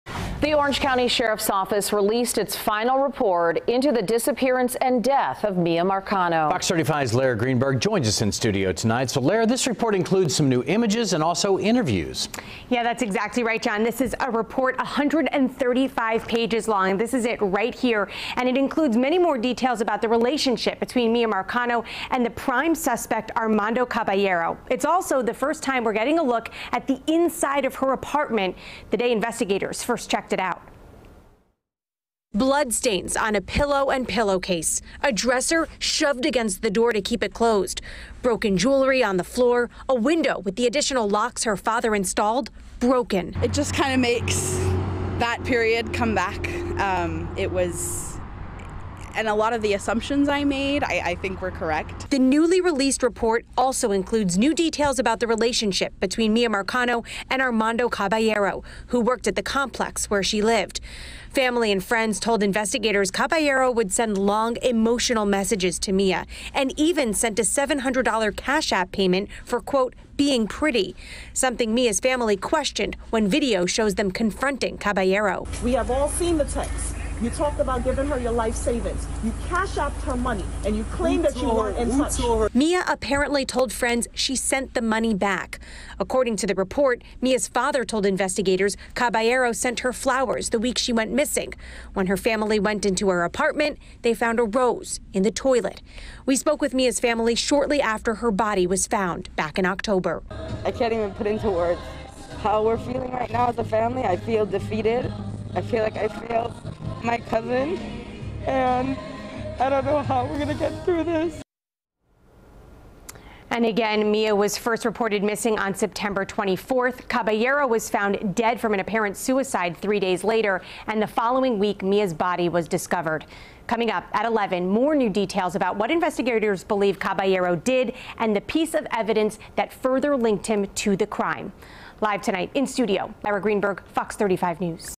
Sheriff's Office releases complete report on Miya Marcano death investigation
The Orange County Sheriff's Office has released it final complete report on the Miya Marcano death investigation. 35 News at 10 p.m.